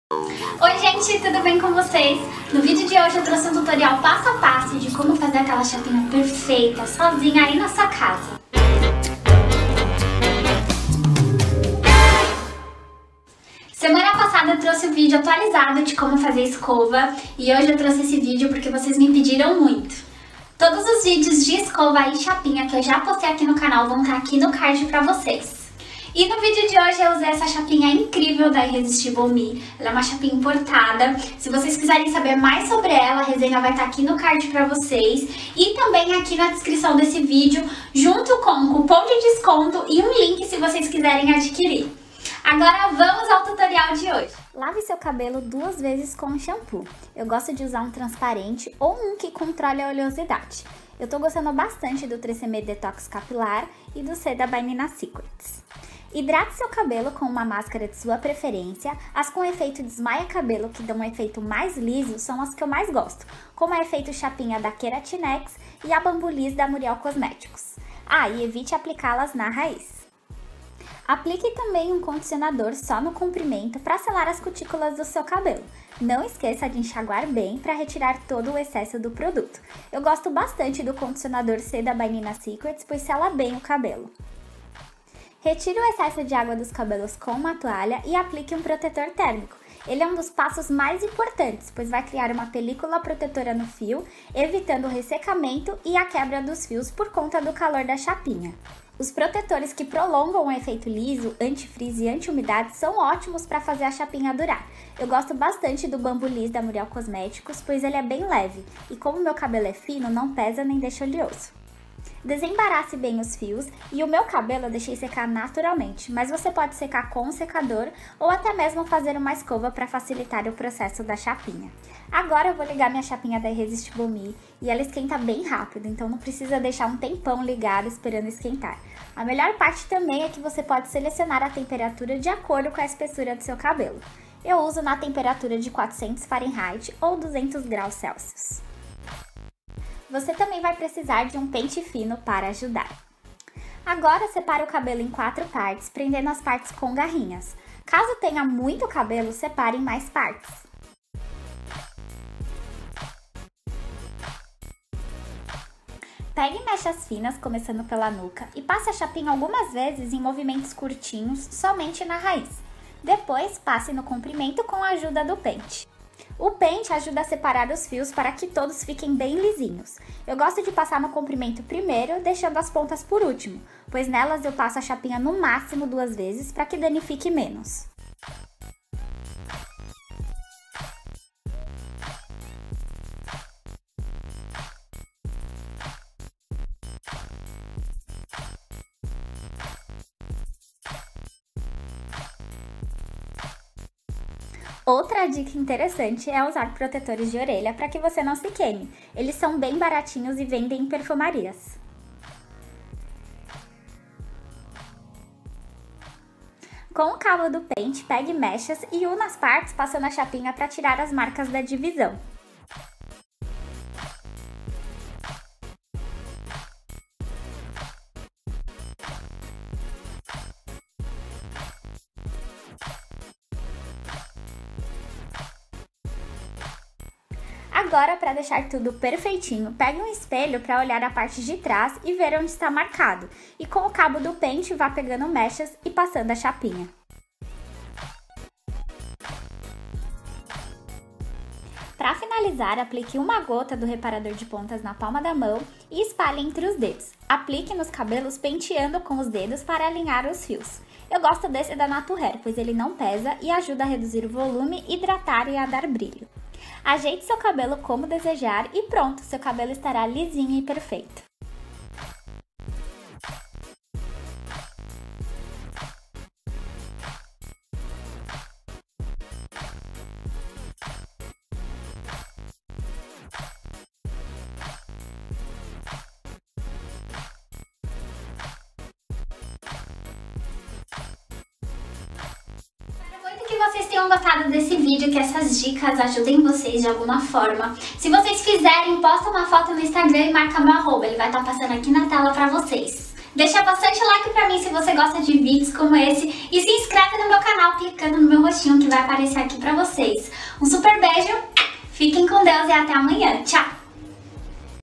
♥ TUTORIAL PASSO A PASSO COMO FAZER CHAPINHA SOZINHA E PERFEITA fevereirotododia 20 ♥
Oiii gente,

Trouxe um vídeo atualizado pra vocês com um passo a passo de como fazer chapinha sozinha e perfeita no cabelo.

Beijos.